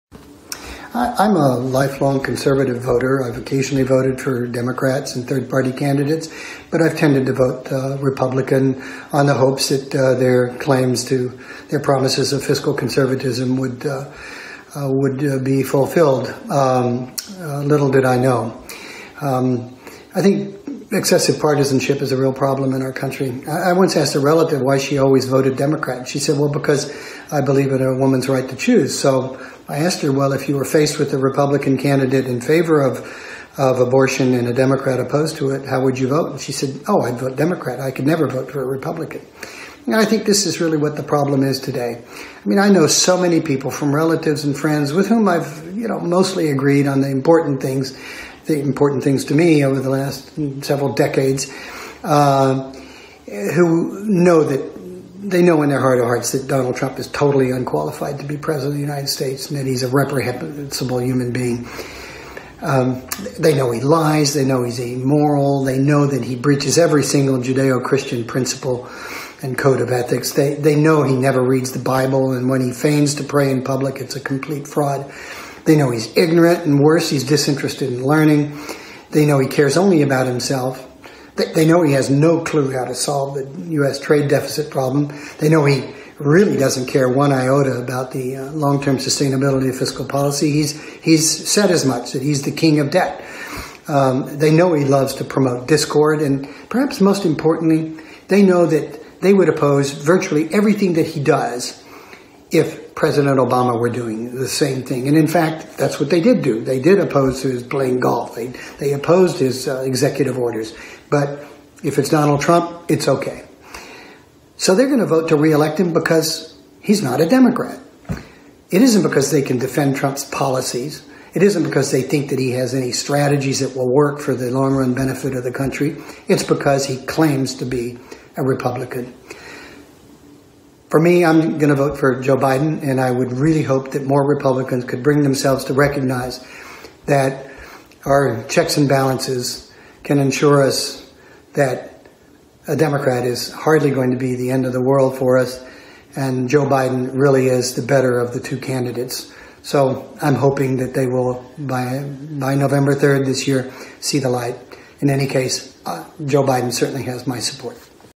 Conservative Voter pleads with Trump supporters to go beyond party in 2020
"I would really hope that more Republicans could bring themselves to recognize...that a Democrat is hardly going to be the end of the world"